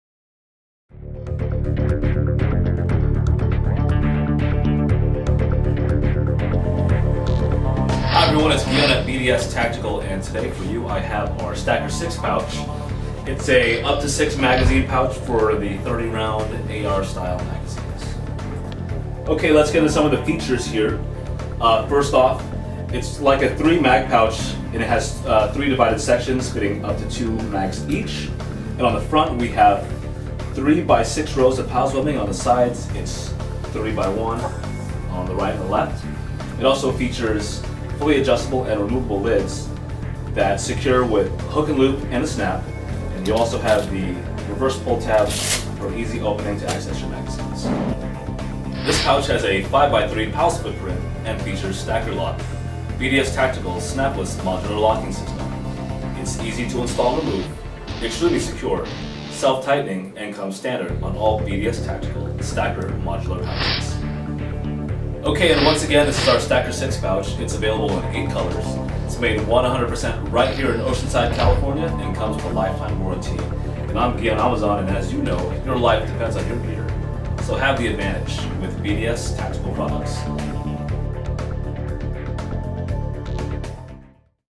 BDS Tactical Modular Stacker 6 Magazine Pouch
The BDS Tactical Modular Stacker 6 Magazine Pouch is made to fit up to 6 GI issue 30 round magazines for the AR-Colt series rifles.  The Stacker 6 pouch is one large pouch with an un-interrupted front that has a divider inside to keep the magazines separate.  The Lids on this magazine pouch are fully adjustable / removable and secures with both hook & loop and a snap.  On the adjustable lid is a 1.5" inverted pull tab for easy access especially when wearing gloves. The front and sides are covered in with 3 rows of PALS webbing for mounting optional modular pouches. There is a large grommet on the bottom of the pouch for excellent drainage. The Stacker 6 Magazine Pouch takes up 5 across, and 3 rows down of PALS webbing and attaches with the BDS Tactical Stacker Lock snapless modular locking system.